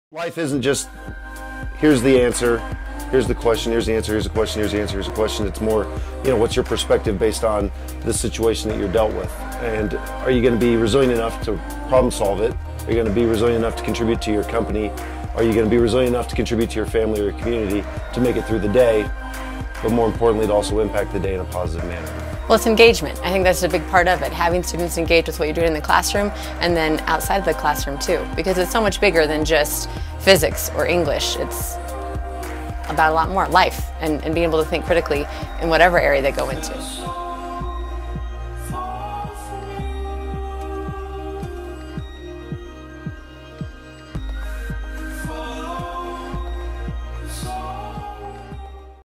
Inside and Out, Episode 3: Problem Solving Matters
In real life, memorization and information regurgitation won’t cut it. Education should be more than that, too. High school students need skills to think critically and be engaged so they can make an impact on the problems they encounter.